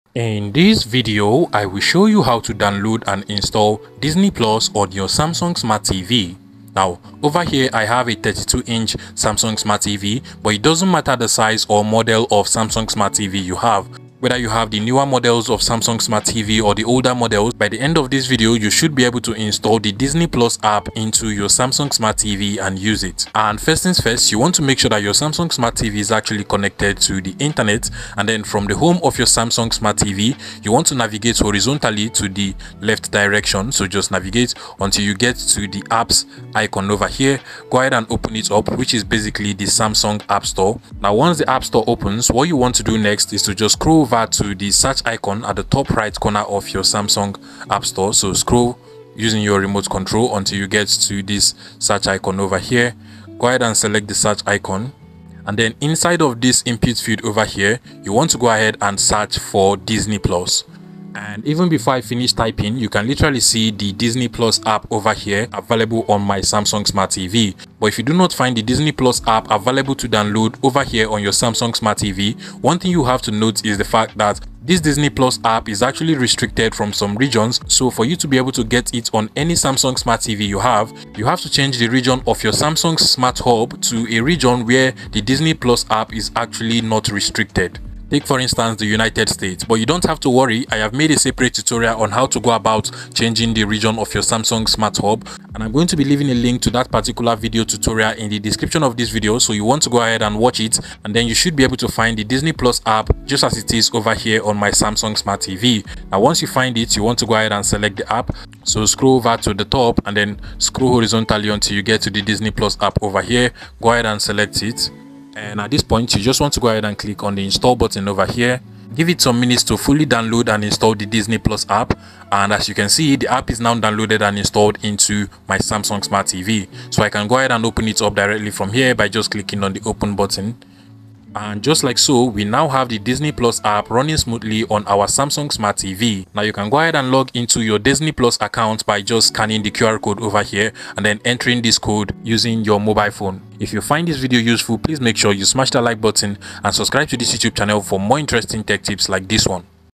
Samsung Smart TV: How to Download and Install Disney Plus
In this video, I’ll show you how to download and install the Disney Plus app on your Samsung Smart TV so you can start streaming your favorite movies and shows. The Disney Plus app is available on the Samsung Smart TV App Store, and I’ll guide you step by step on how to access the store, search for the app, and install it without any issues.

If you can’t find Disney Plus on your Samsung Smart TV, that means the app isn’t available in your region. Don’t worry—I’ve got a solution for that too. I’ll show you how to change your Samsung Smart TV region so you can get the Disney Plus app and start enjoying all its content. Whether you’re setting up Disney Plus for the first time or troubleshooting installation issues, this tutorial has you covered.

Check out this video to Change the Region of your Samsung TV to make the Disney plus app available